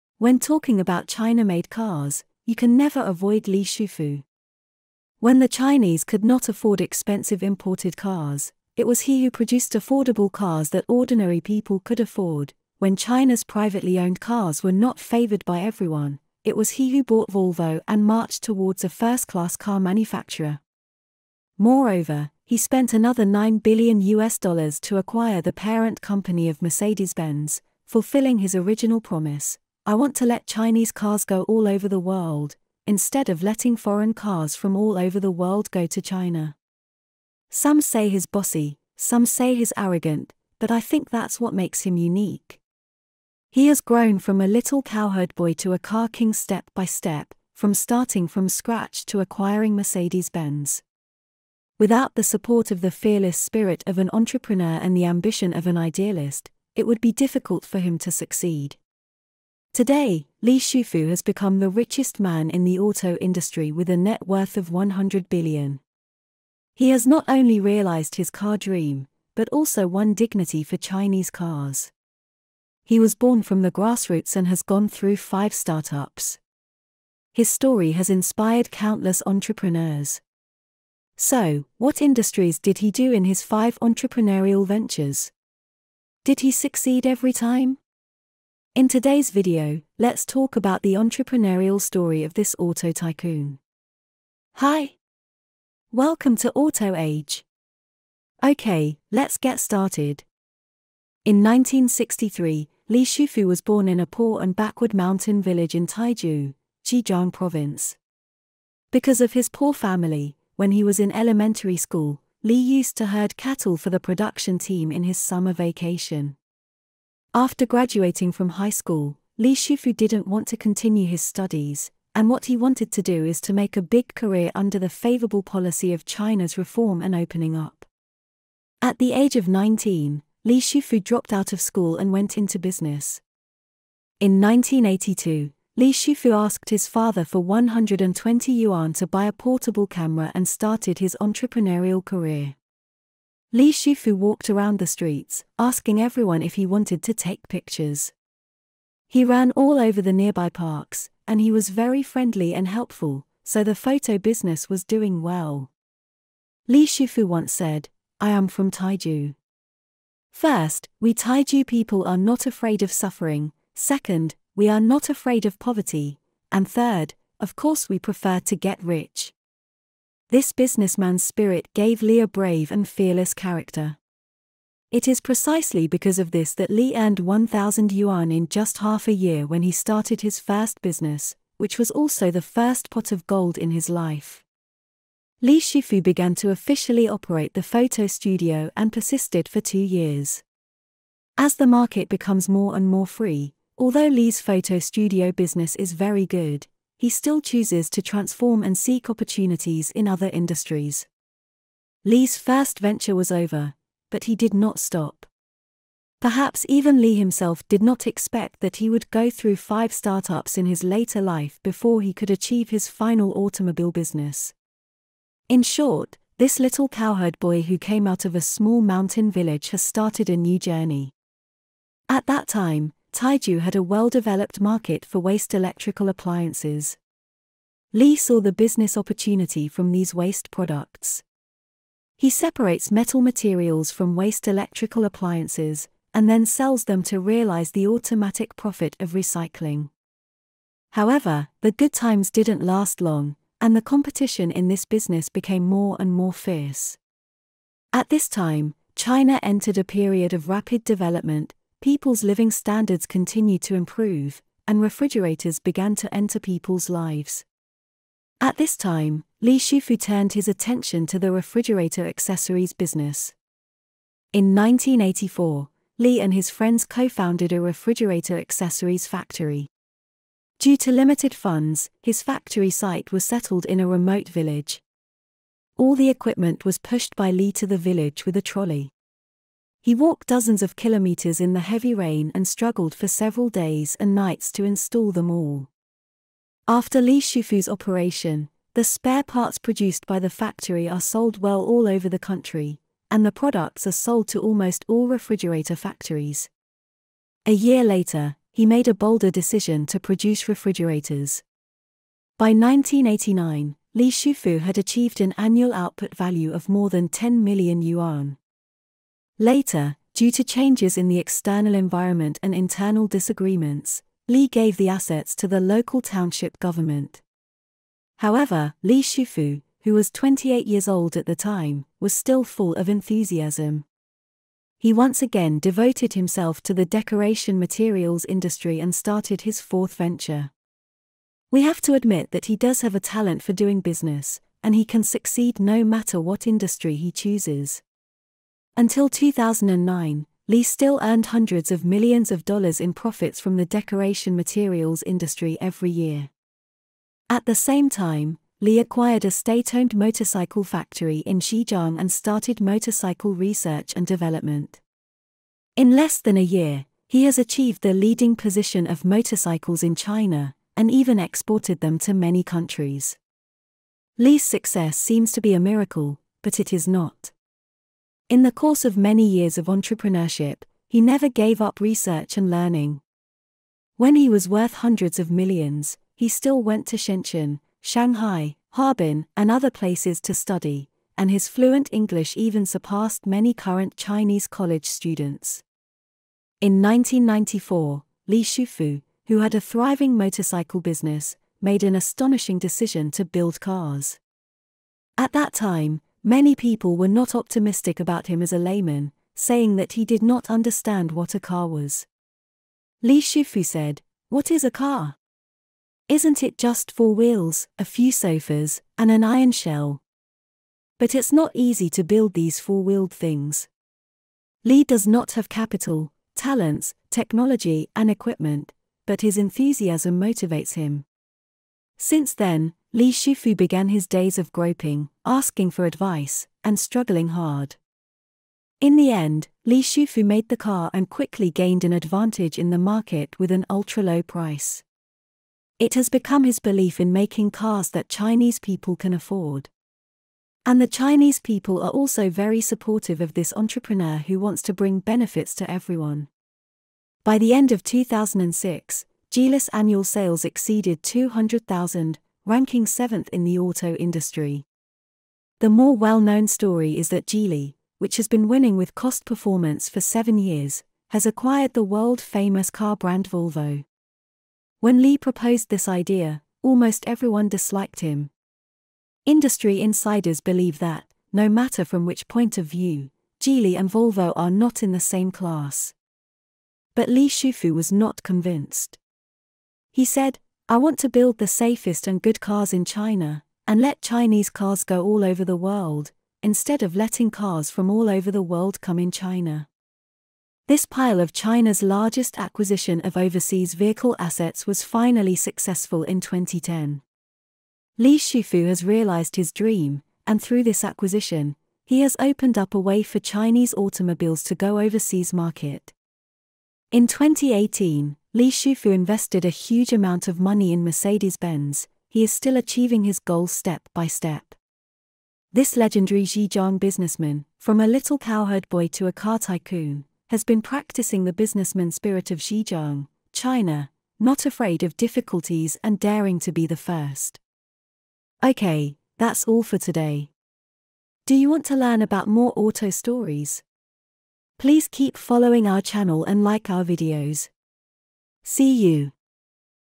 From a cowherd boy to a car king, he is determined to let China make cars go overseas!
When talking about China-made cars, you can never avoid Li Shufu. When the Chinese could not afford expensive imported cars, it was he who produced affordable cars that ordinary people could afford; when China’s privately owned cars were not favored by everyone, it was he who bought Volvo and marched towards a first-class car manufacturer. Moreover, he spent another US$9 billion to acquire the parent company of Mercedes-Benz, fulfilling his original promise: "I want to let Chinese cars go all over the world, instead of letting foreign cars from all over the world go to China."

Some say he's bossy, some say he's arrogant, but I think that's what makes him unique. He has grown from a little cowherd boy to a car king step by step, from starting from scratch to acquiring Mercedes-Benz. Without the support of the fearless spirit of an entrepreneur and the ambition of an idealist, it would be difficult for him to succeed. Today, Li Shufu has become the richest man in the auto industry with a net worth of 100 billion.

He has not only realized his car dream, but also won dignity for Chinese cars. He was born from the grassroots and has gone through 5 startups. His story has inspired countless entrepreneurs. So, what industries did he do in his five entrepreneurial ventures? Did he succeed every time?
In today's video, let's talk about the entrepreneurial story of this auto tycoon. OK, let’s get started!